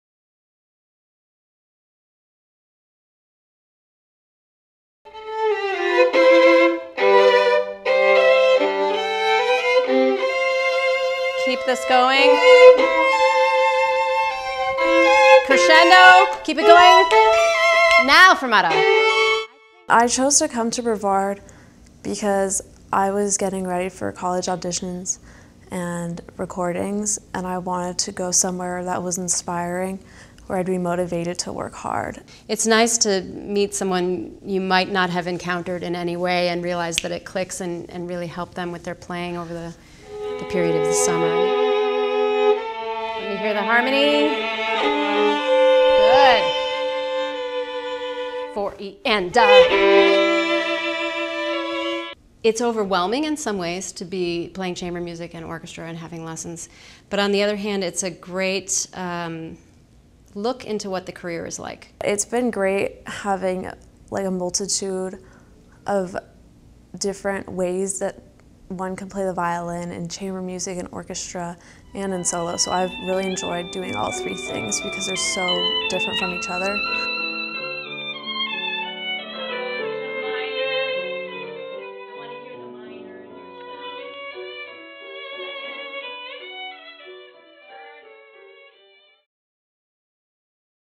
Carolyn Huebl & Sandra Bouissou: Violin @ BMC
Be inspired by a summer at Brevard Music Center! Watch and listen as violinists Carolyn Huebl (BMC Violin Faculty) and Sandra Bouissou (2016 BMC Alumna, High School Division) share insights and lessons learned. Be here to experience solo, chamber, and orchestral performances in one summer.

Video credit: Henry Felt